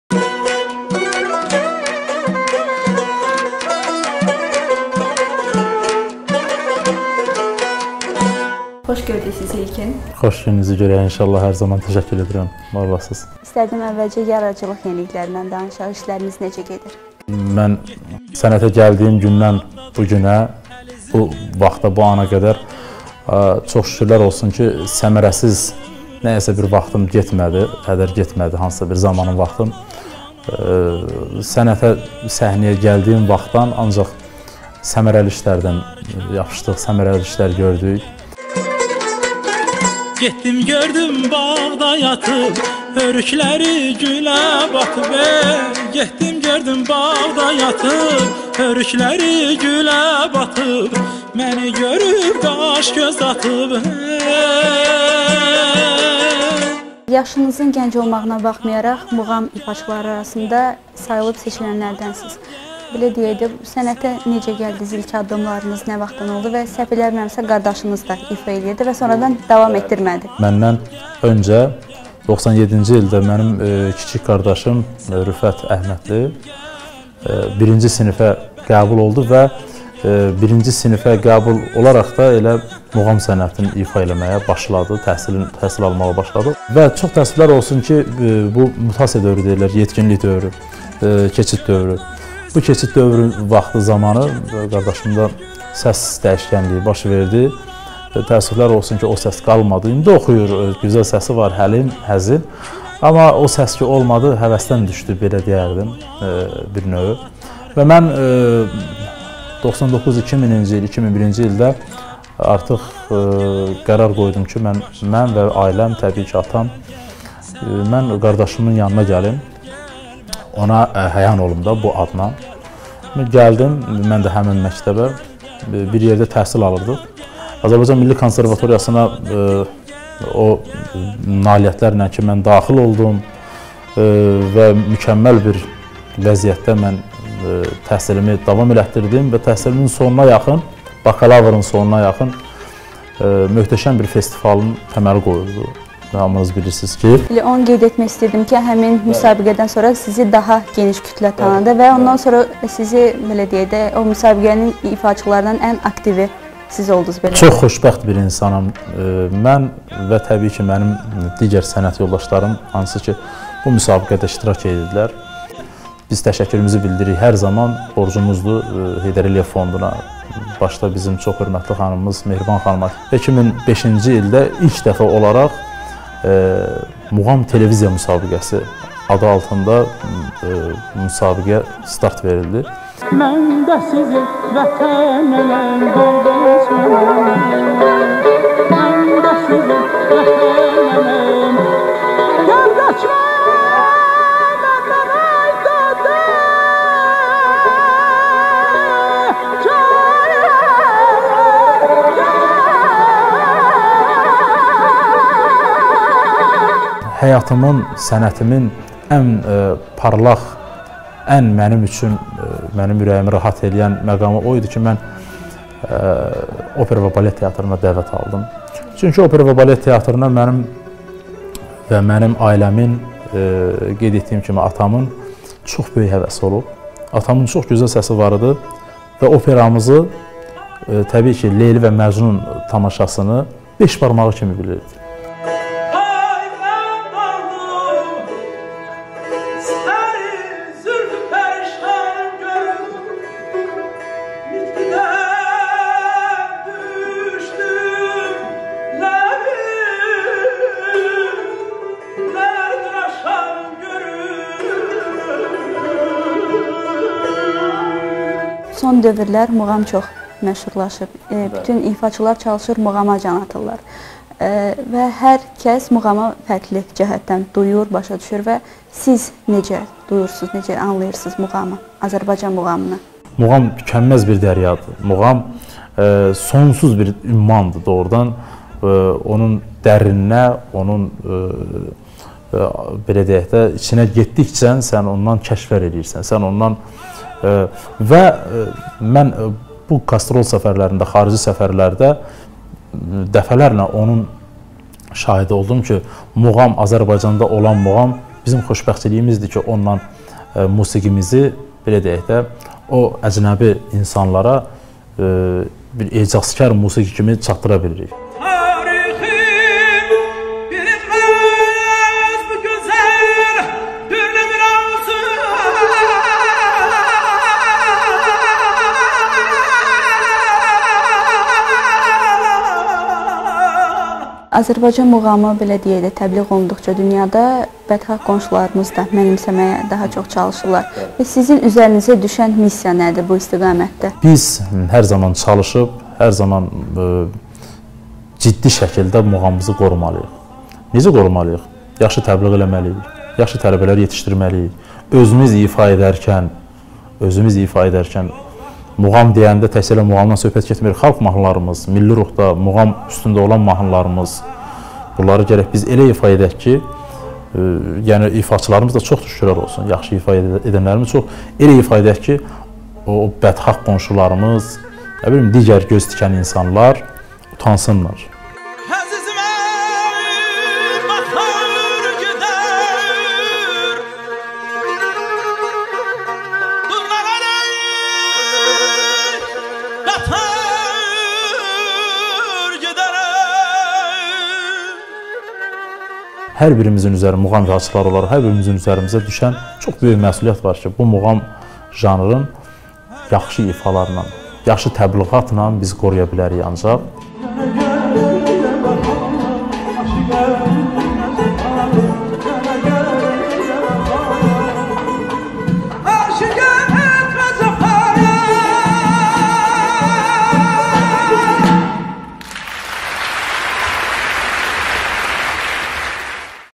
İlkin Əhmədov: Muğamı yaxşı ifalar və təbliğatla qoruya bilərik
Əməkdar Artist İlkin Əhmədov AZƏRTAC-a müsahibəsində ailədə ilk olaraq kiçik qardaşının sənətə gəldiyini dedi. Xanəndə, qardaşının səsində sonradan dəyişiklik baş verdiyindən muğamdan uzaqlaşdığını bildirdi...